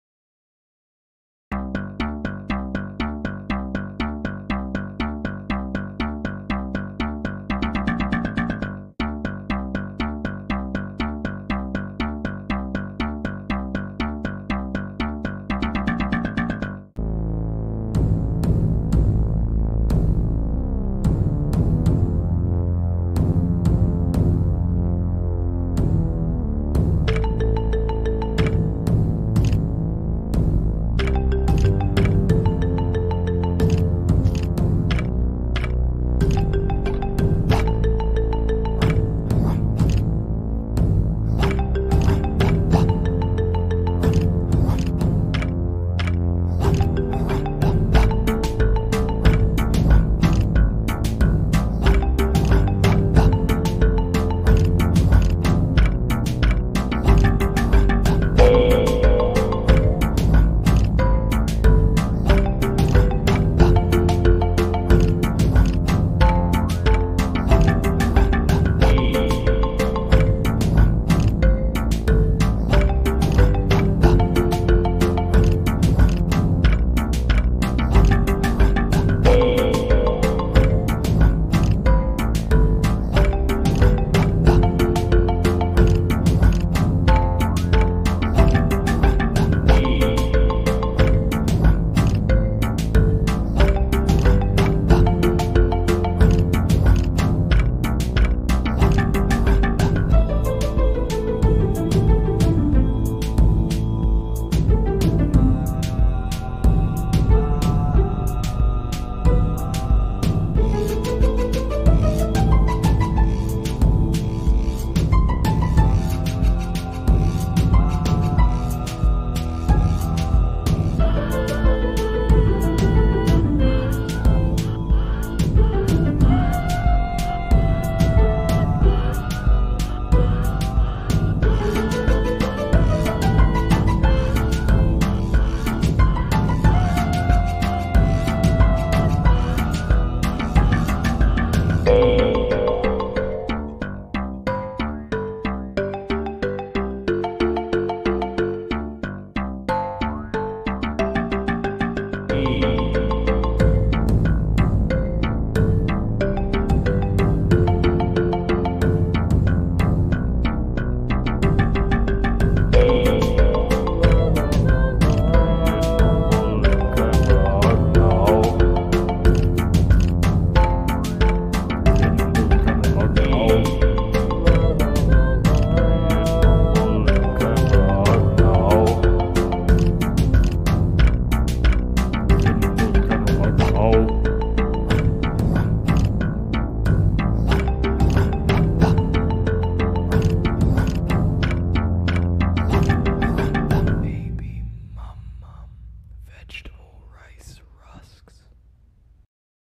Not so daily character 56 1/2 Neann 2
Chadford island series
Neann 2: the new and improved Neann built by genius cat is genius just like them. Although It doesn't look different, It's molecular structure has changed. It is pure metal. using its new metal body it can produce a slap bass sound, similar to Neann's upright bass.

Chadford and the Bradford islands by me99999